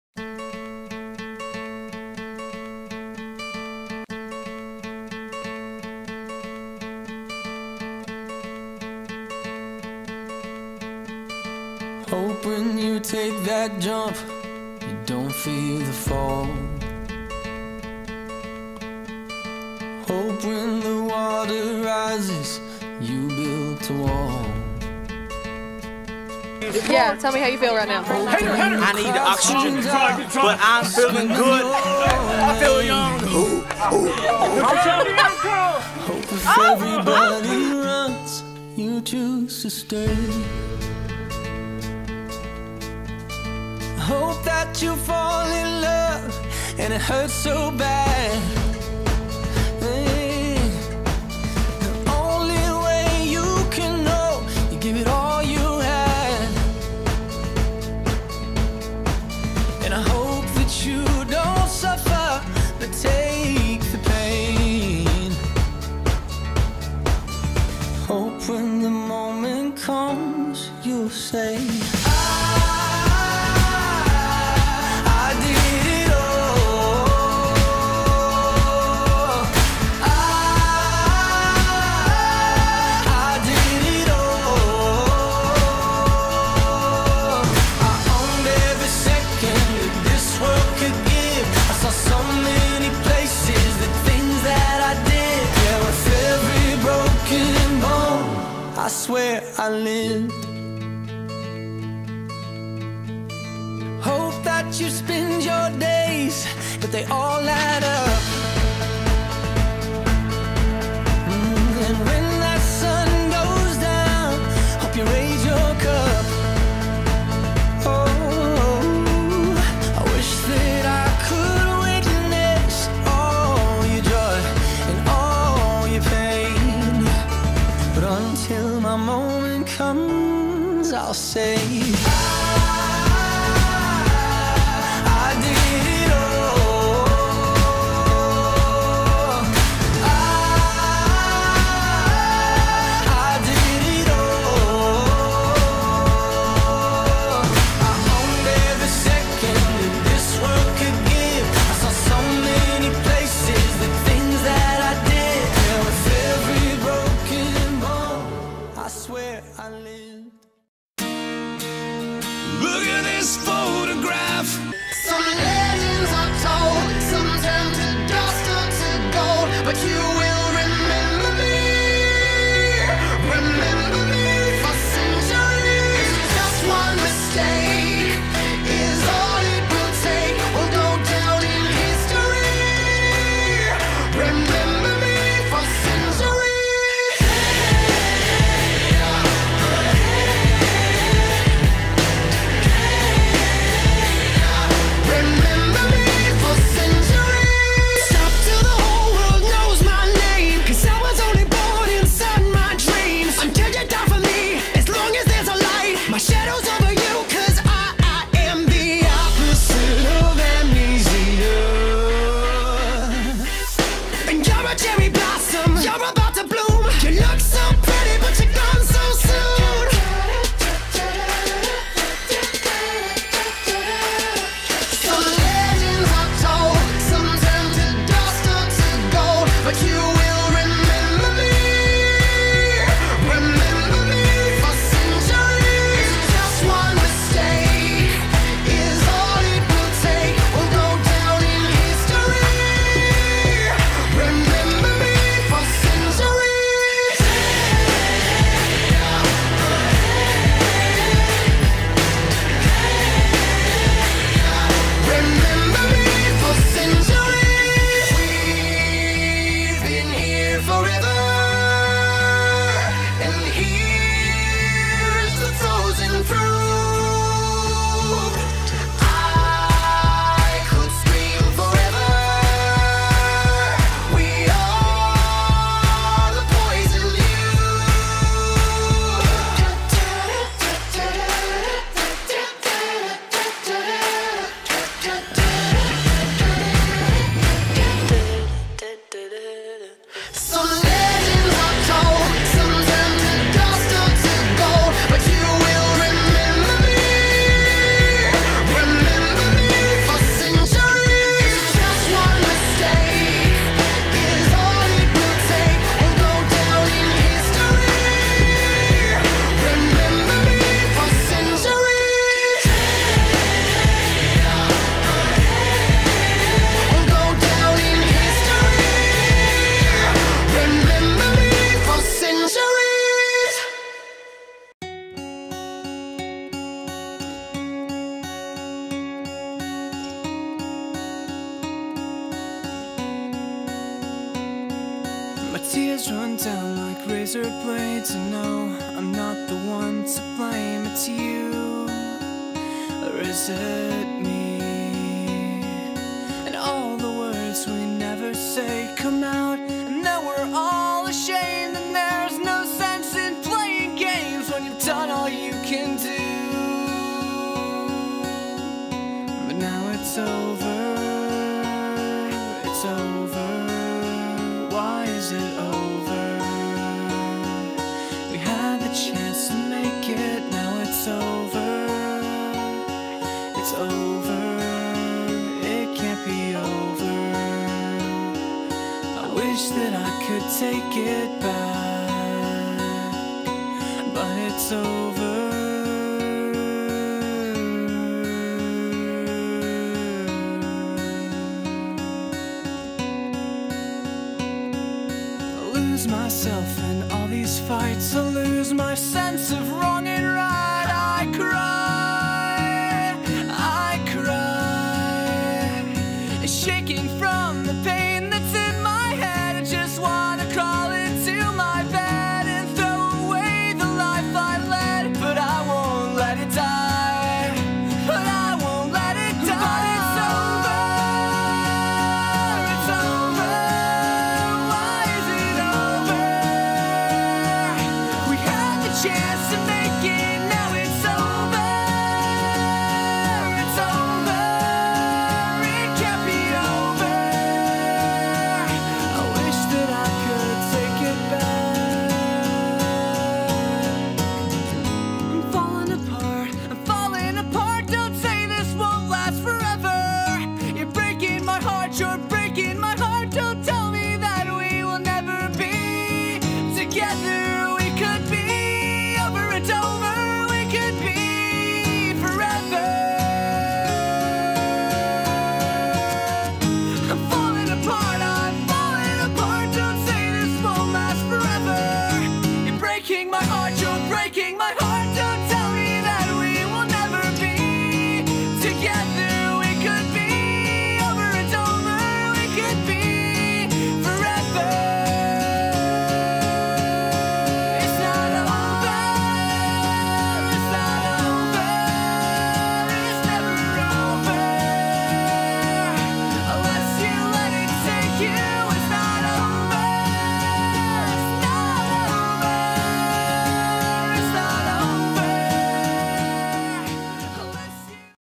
Faulkner University Study Abroad-Spring 2015 Montecatini, Italy
A brief recap of an epic semester abroad!